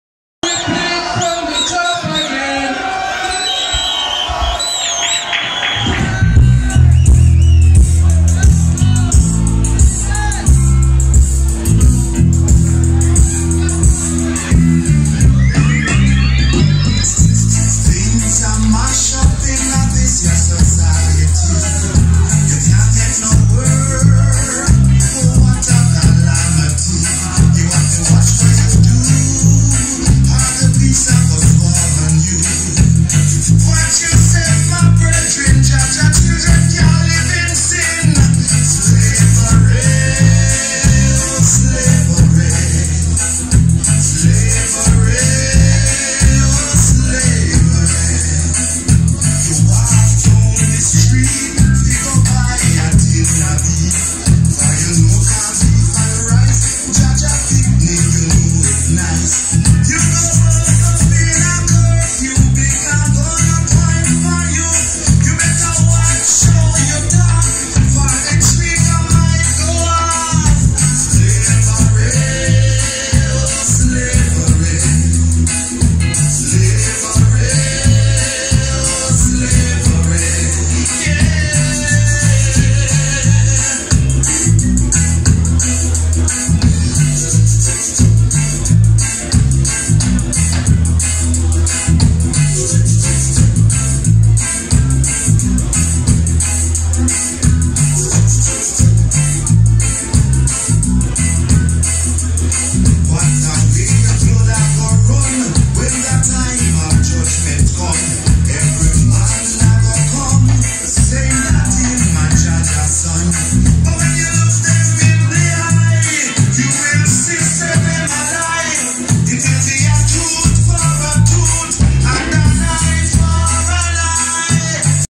Rootikal Vibes HiFi @ Alp'in'Dub 2019 - Lloyd Charmers
Alp'in'Dub Festival 23/08/2019 22:42
Keety Roots
Vibronics
Agobun Sound System 8 scoops
Rootikal Vibes HiFi 8 scoops
Lloyd Charmers selection
Thanks Loona for the video footage